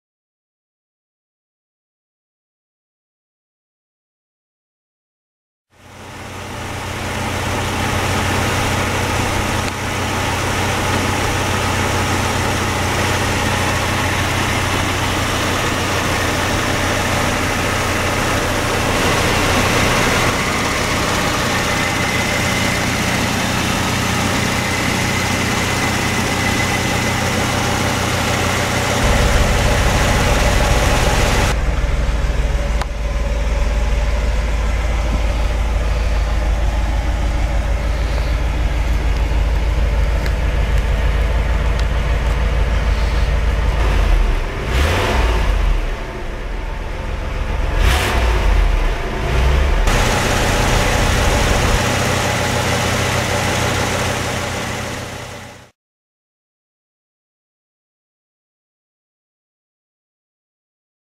1968 Lincoln Lehmann Peterson Limo Engine Sound
to see this great classic 1968 Lincoln Continental Lehmann Peterson Limousine for sale from Left Coast Classics in Sonoma California.

Truly the most beautiful I have seen or had in my inventory. Finished in dark metallic blue with deep blue leather interior. Owned for many years by a gentleman in Illinois who took an already very nice and original car and spent in excess of $80,000 to get it to this beautiful condition. It was repainted in it's original blue, given a new vinyl top, new chrome, polished stainless and plenty of new rubber. The car is in wonderful cosmetic condition.

The interior has new leather, carpets a beautiful dash, clear glass and perfect headliner. The features function mostly as intended though at the time of photographing the car I noted that the front ashtray would not stay closed. Otherwise, climb inside and you'll find lighting, horn, radio, antenna - all in working order as are the gauges. Clock(s) rebuilt with quartz. The trunk has been completely restored with all of the proper panels, rugs, jack and spare. It's immaculate through and through.

Take a look at the full description as well as 2 detailed photo galleries at the link above then give me a call to see her in person.